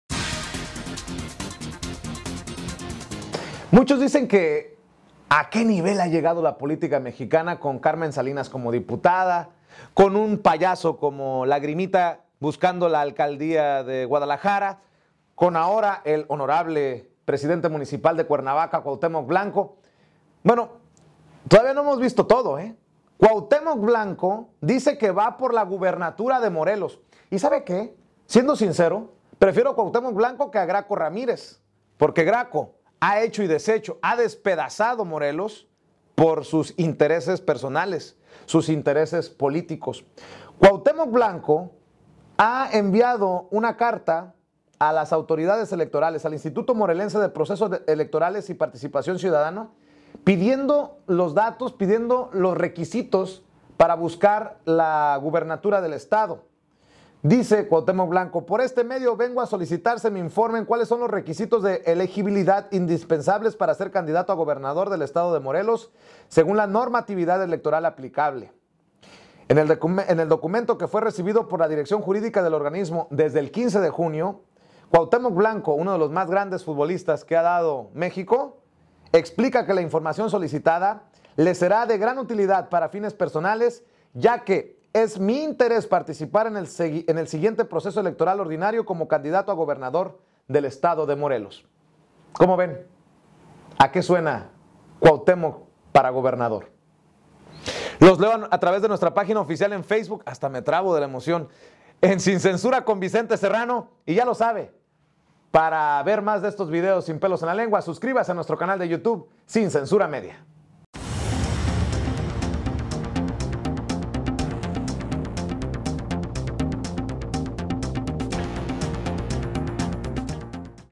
El “Cuau” ahora quiere la gubernatura de Morelos; pero entre Cuauhtémoc y Graco Ramírez...
El exfutbolista y alcalde de Cuernavaca, Cuauhtémoc Blanco Bravo, solicitó de manera oficial al órgano electoral estatal los requisitos para ser Gobernador del estado.